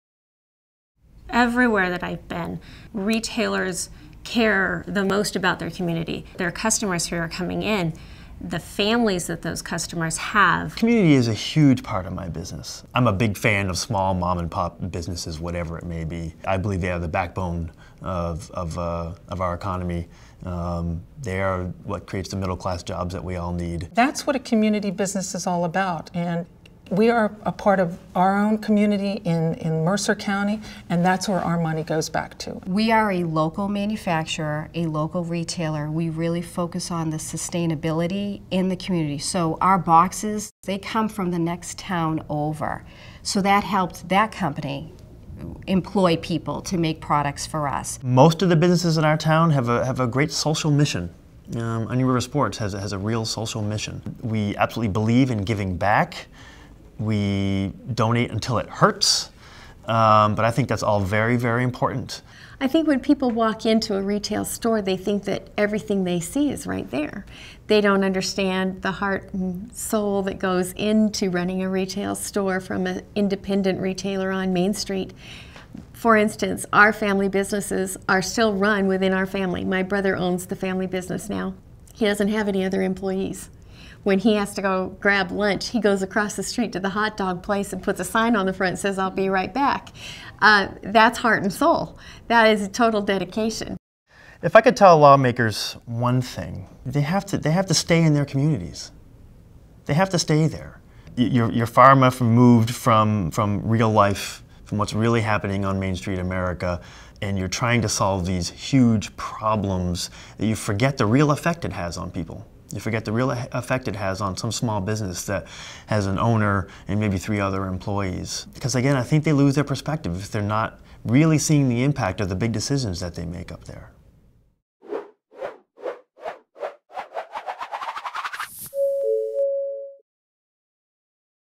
Retail is committed to communities
Retail business owners and employees share their thoughts on the community impact retail business has in their towns, and the effect policymakers' decisions have on the local economy back home.